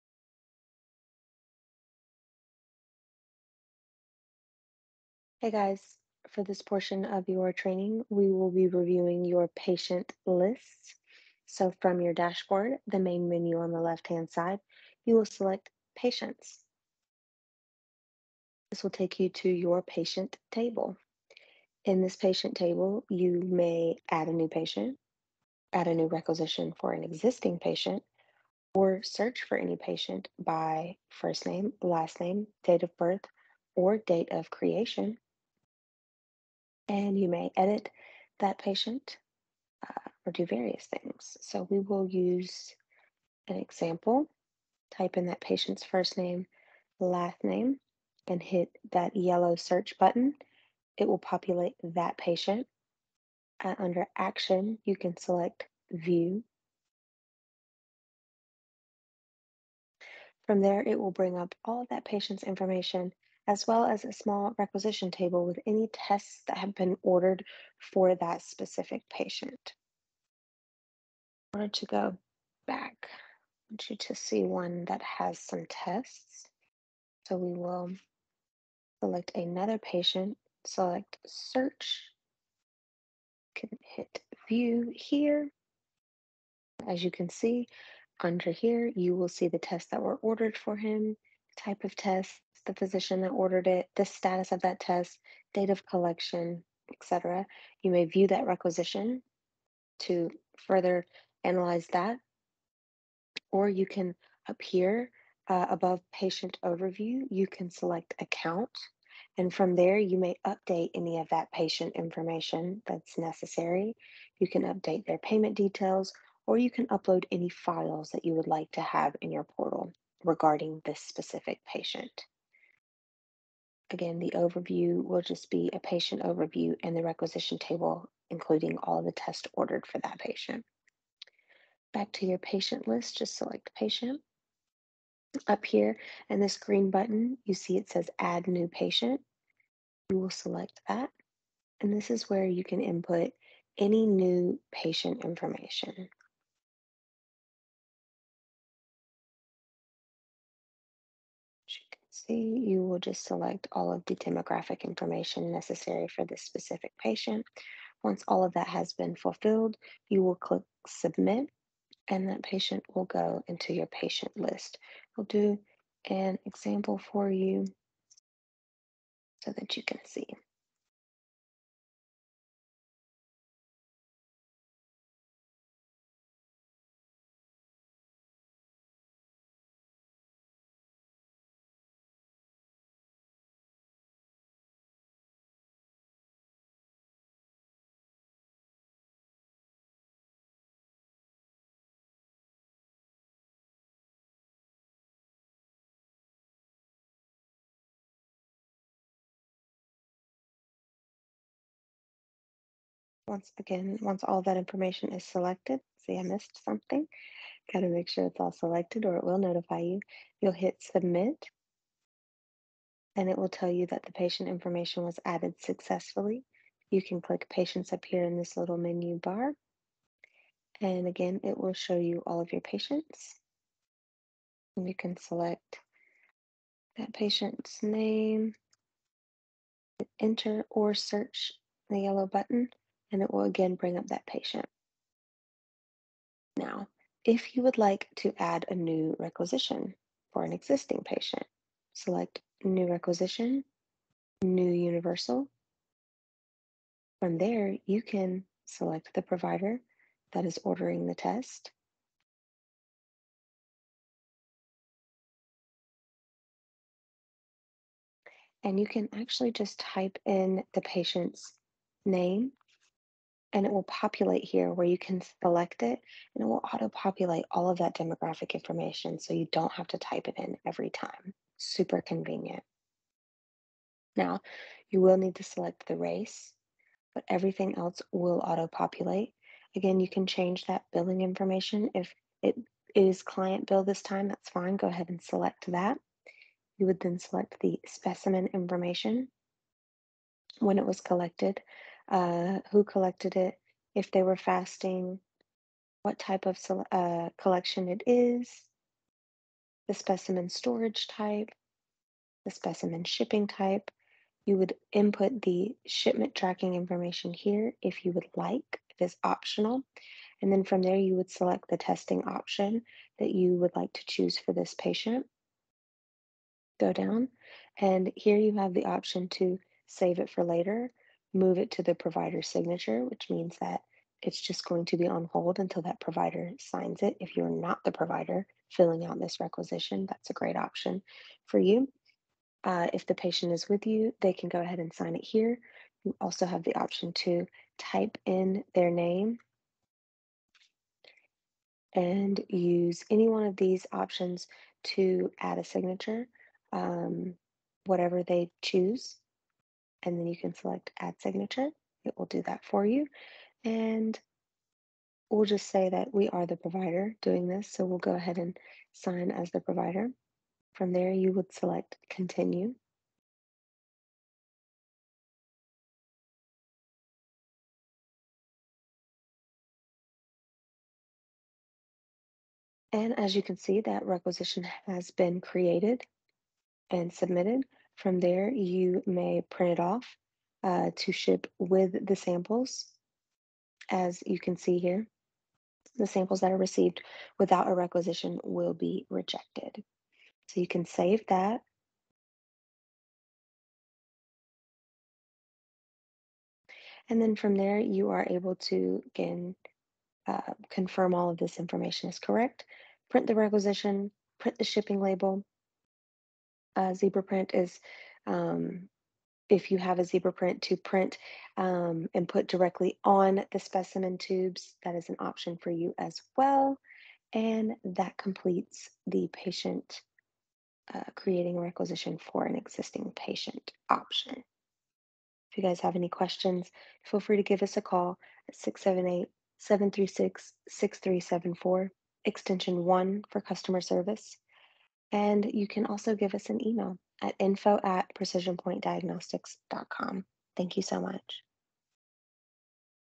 Patient List in your Precision Point Diagnostics Provider Portal
This video explains the Patient List in your Precision Point Diagnostics Provider Portal.